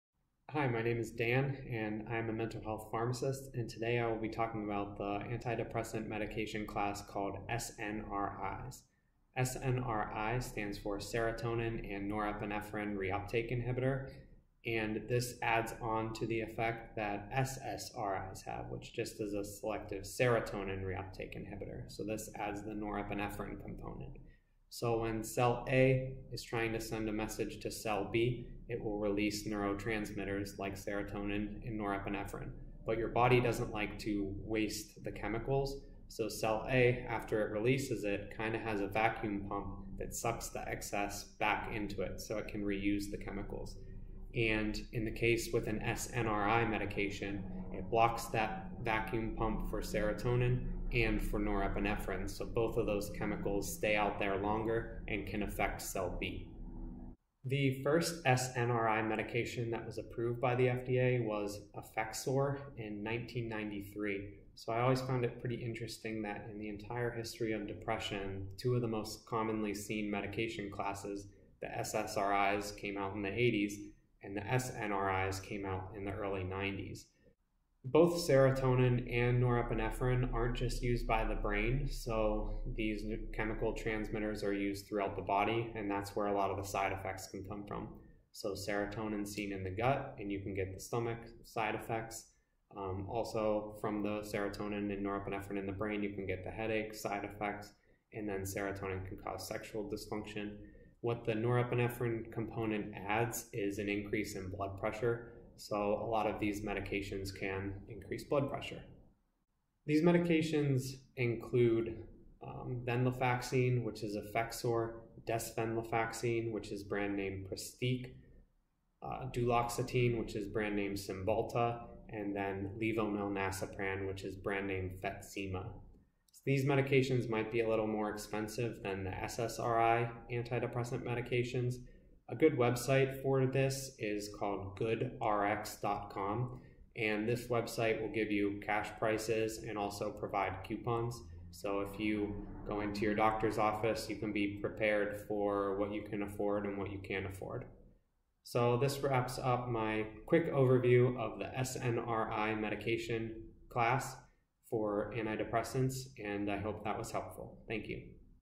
SNRI Antidepressant Overview
This is an overview of the SNRI antidepressant medication class.

Pharmaceuticals should never be used as the sole treatment for mental illnesses. Therapy, exercise, meditation or other treatments should always accompany prescriptions.